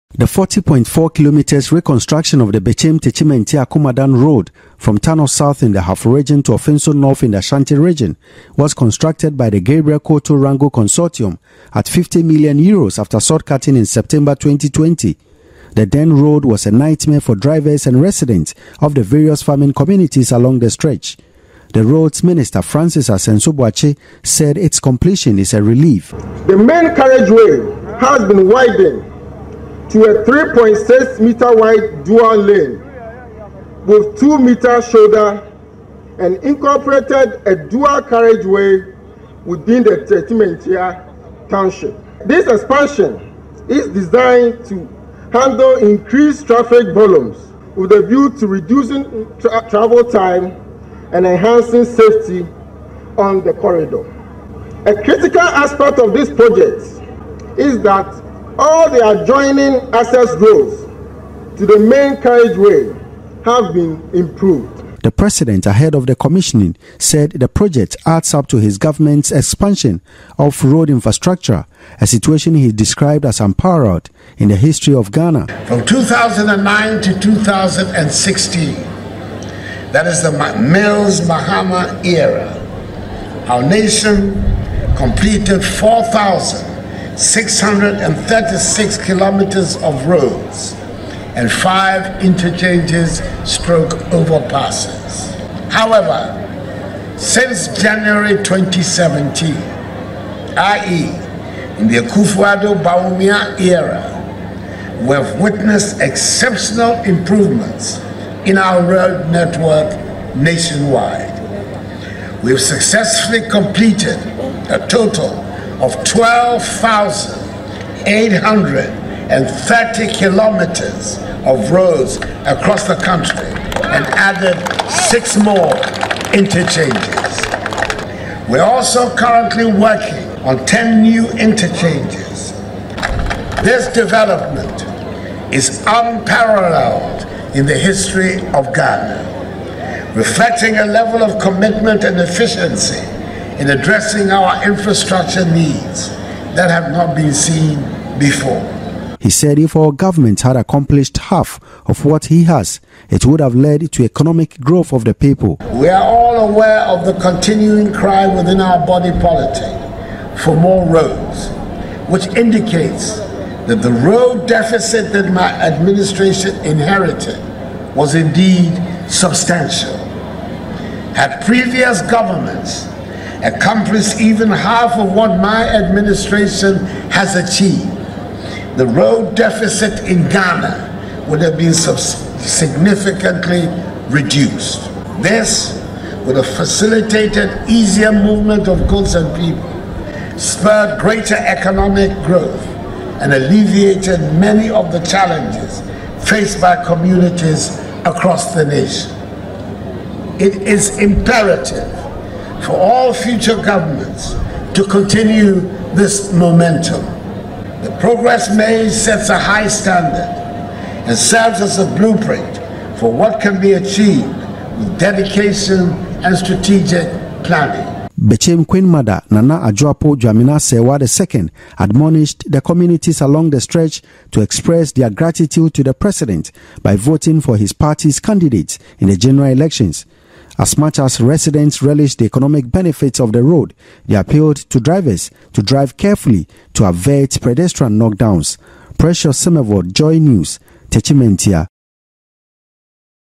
Akufo-Addo commissions 50M Euros 40.4km Bechem-Techimantia-Akomadan road
Nana Akufo-Addo described the construction of 12,830 km of roads and 6 interchanges under his administration as unparalleled in the history of Ghana's road infrastructure development.